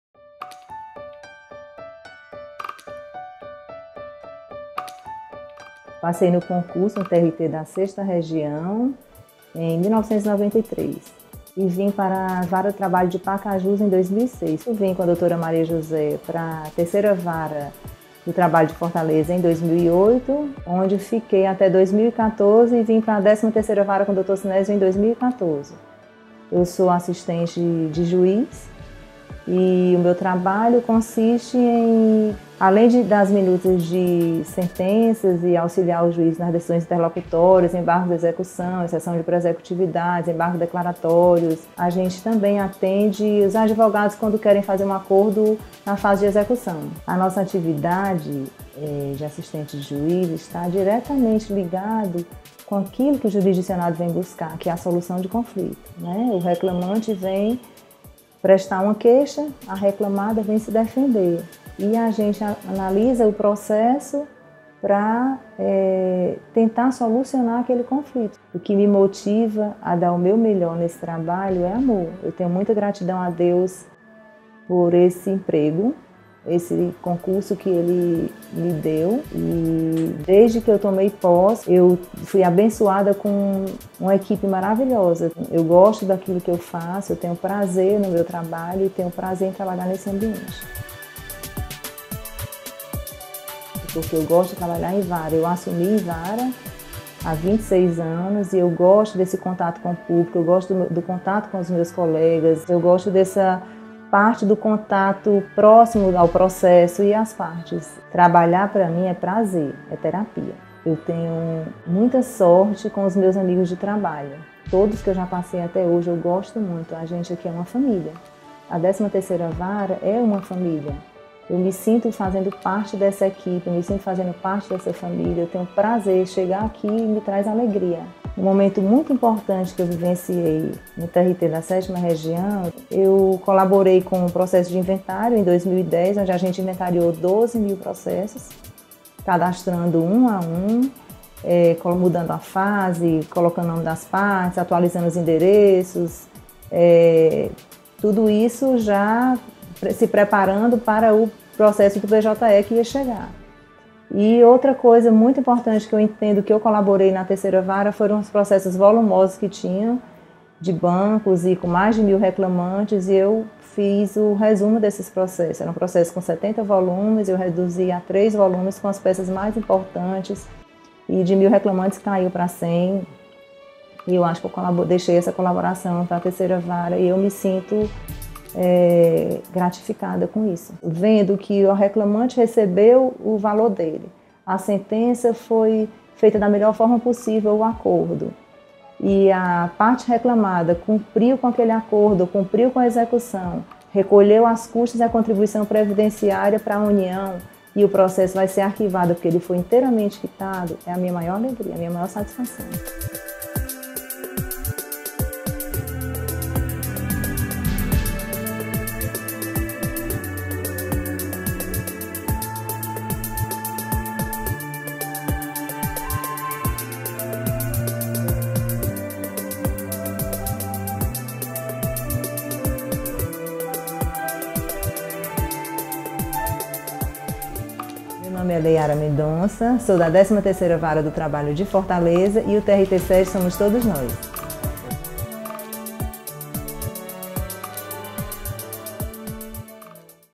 O TRT7 Somos Todos Nós – Leyara
"O TRT7 somos todos nós" é uma campanha da Gestão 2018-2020 da Presidência do TRT da 7ª Região cujo objetivo é a valorização dos servidores. A série de vídeos apresenta um servidor por mês, que conta suas motivações, desafios e realizações em seu ambiente de trabalho.